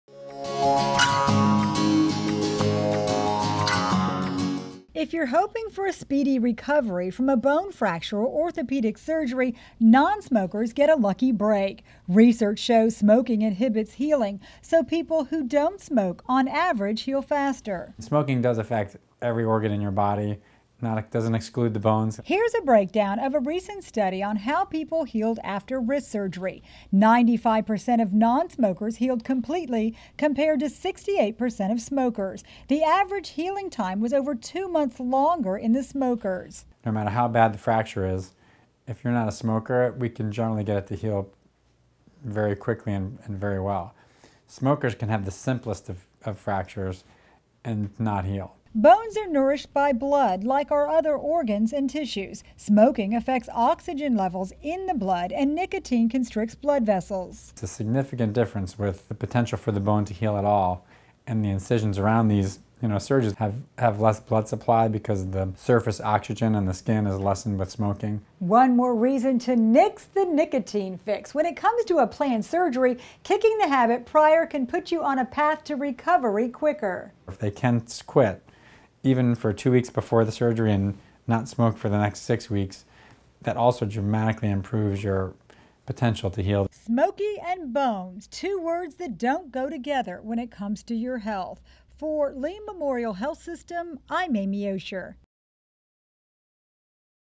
Smoking Affects Bone Healing
If you’re hoping for a speedy recovery from a bone fracture or orthopedic surgery, non-smokers get a lucky break. Research shows smoking inhibits healing. People who don’t smoke, on average heal faster.

“Smoking affects every organ in your body,” says orthopedic surgeon Dr. Ed Humbert. He is on medical staff of Lee Memorial Health System. “that doesn’t exclude bones.”

Here’s a breakdown of a recent study on how people healed after wrist surgery.  95% of the non-smokers healed completely, compared to 68% of smokers. The average healing time was over two months longer in the smokers.

“No matter how bad the fracture is, if you’re not a smoker we can generally get it to heal very quickly and very well. Smokers can have the simplest of fractures and not heal,” Dr. Humbert says.

Bones are nourished by blood like our other organs and tissues. Smoking affects oxygen levels in the blood and nicotine constricts blood vessels.

“It’s a significant difference with the potential for the bone to heal at all. Along with the incisions around these surgeries. We have less blood supply because the surface oxygen level is lessened by smoking.”

Just one more reason to nix the nicotine fix. When it comes to a planned surgery- kicking the habit prior can put you on the path to recovery quicker.

“If they can quit even two weeks before the surgery and then not smoke for the next six weeks, that also dramatically improves potential for healing,” Dr. Humbert says.

Smoky and bones- two words that don’t go together when it comes to your health.